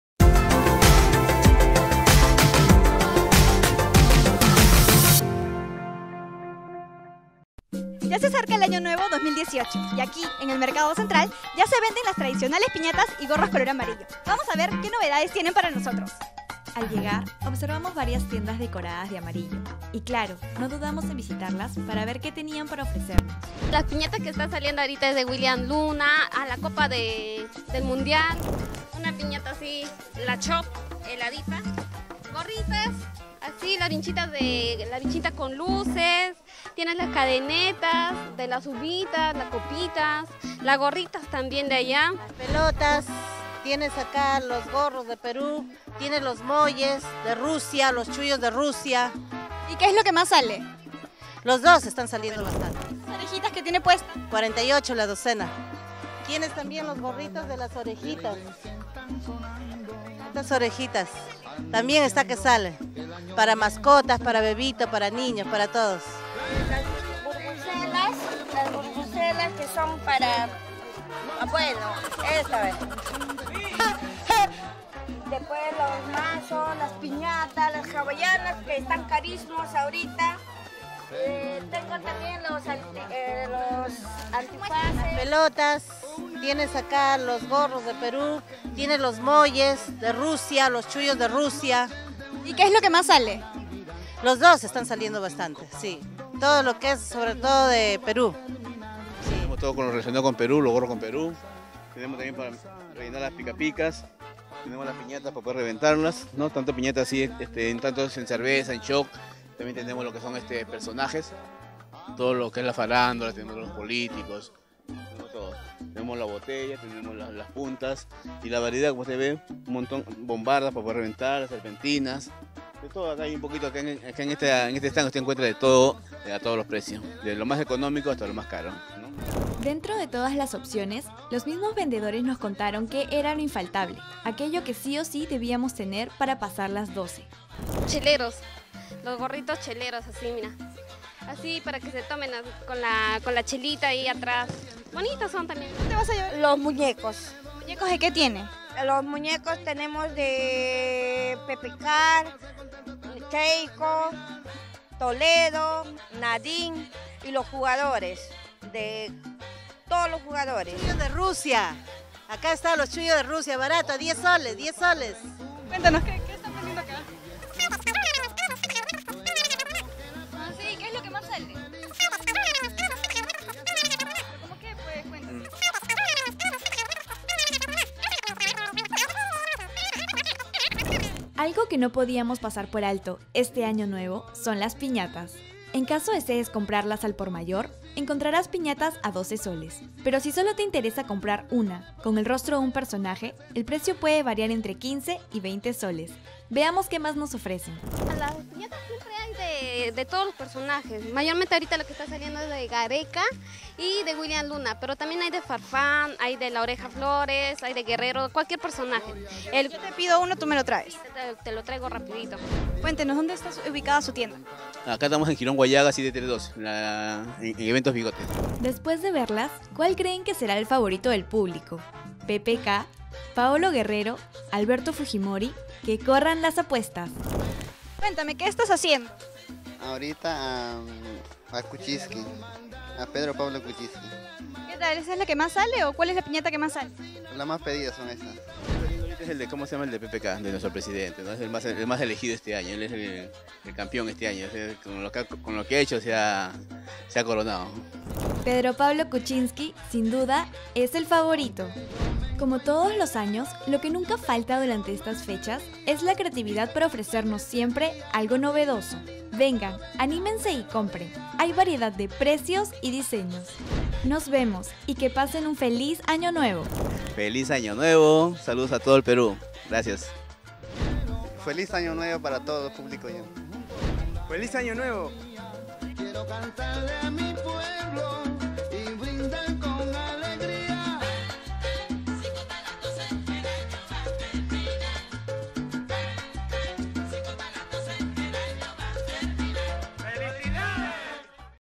Novedades para recibir el 2018
Se acerca el final del año, y es la oportunidad perfecta para realizar un recorrido por las calles del mercado central con la finalidad de conocer que novedades tienen los comerciantes para despedir el 2017 y recibir a todo dar, el 2018.

Informe: Melanie Cairo / Imágenes: Ivan Medina - Michael Asencios / Edición: Lino Montes